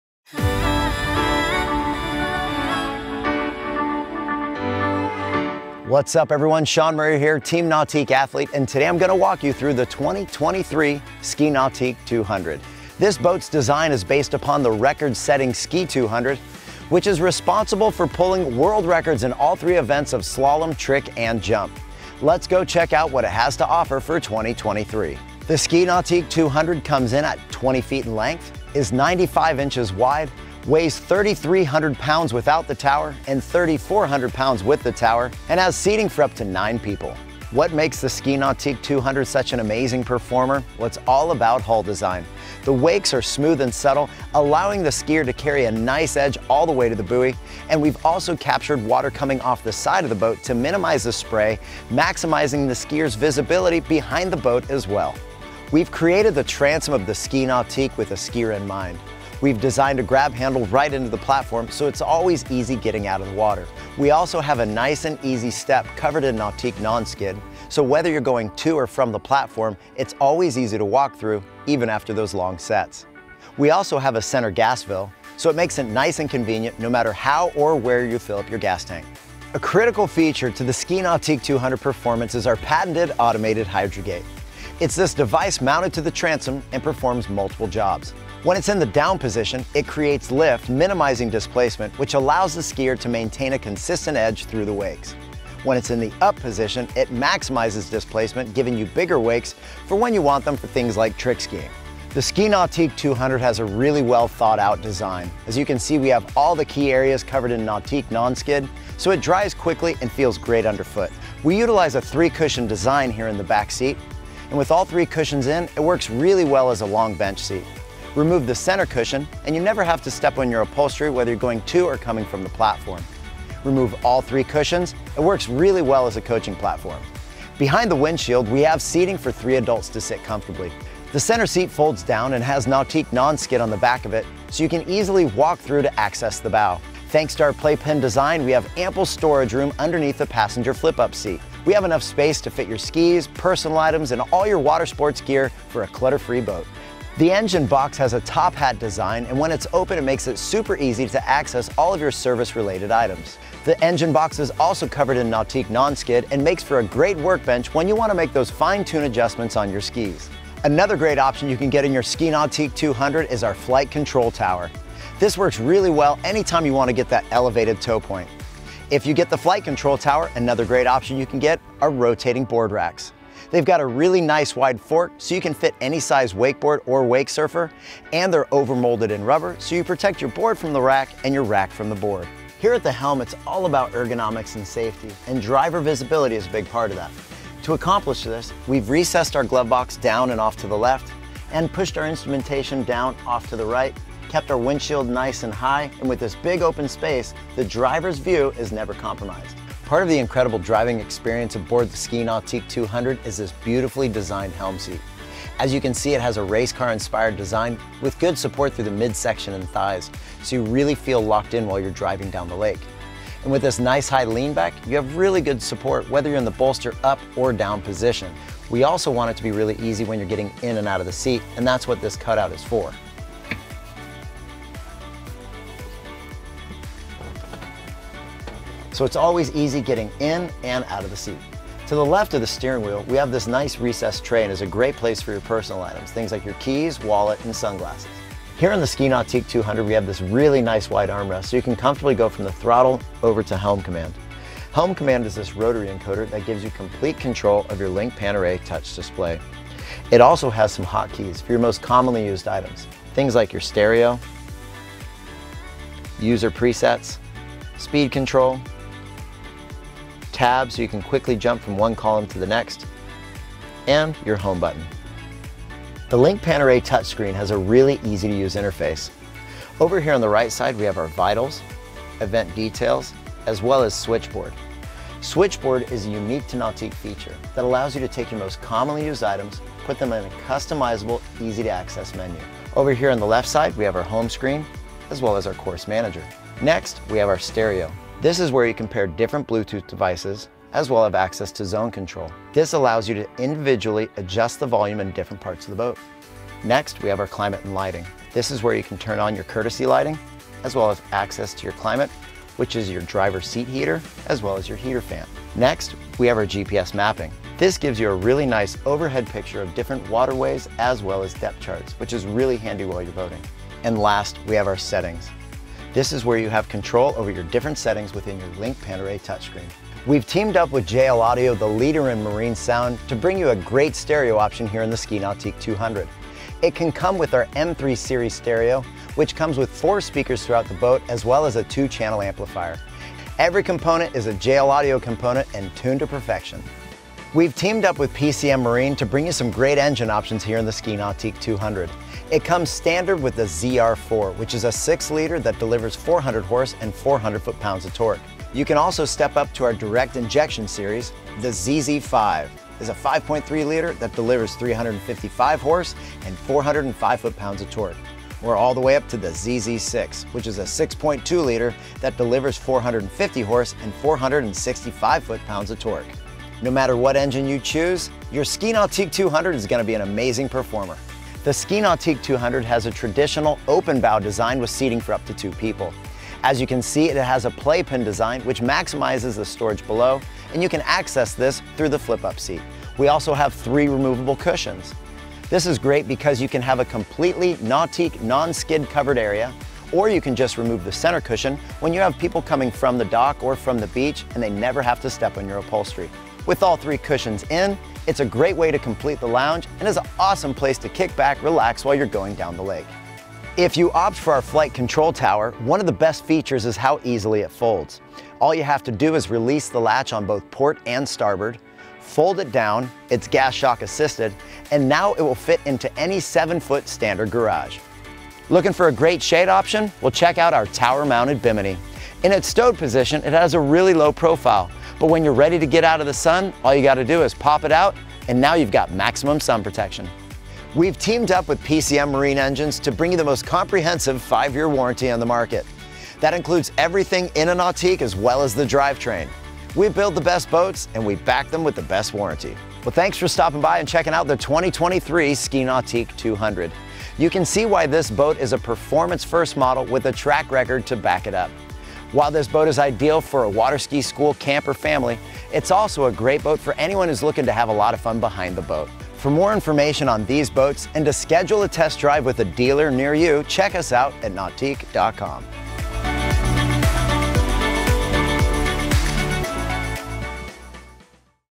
2023 Ski Nautique 200 Walk Through
2023 Ski Nautique 200 Walk Through. Created upon one of the most decorated hulls in waterski history, the Ski Nautique 200 is a performance-first model with the track record to back it up. Built off the framework of the world-record setting Ski 200, this model is an AWSA tournament-approved towboat that produces soft wakes for all levels of waterskiing. With an open interior layout and a playpen-style bow, the Ski Nautique 200 is a traditional bow ski boat that speaks to the Nautique purist in all of us.